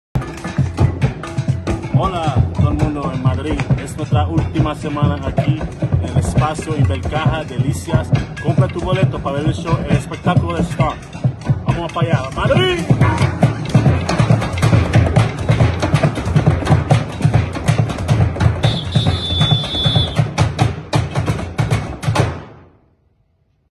ÚLTIMA SEMANA de STOMP en MADRID
STOMP
Del 4 al 21 de mayo
La explosiva combinación de percusión, música, humor y danza que ha convertido Stomp en un verdadero fenómeno global regresa a Madrid tras años de ausencia. Desde su estreno hace casi 25 años ya ha sido visto por más de 15 millones de espectadores en todo el mundo. STOMP fue creado por Lucas Cresswell y Steve McNicholas en Brighton en 1991.

STOMP es el legendario espectáculo que viene haciendo vibrar al mundo con sus energéticas representaciones. Cerillas, escobas, cubos de basura, mecheros y todo tipo de artilugios urbanos llenan el escenario para crear los beats en STOMP. Un espectáculo energizante que mezcla danza, música y performance para dejar electrizados a espectadores de todas las edades.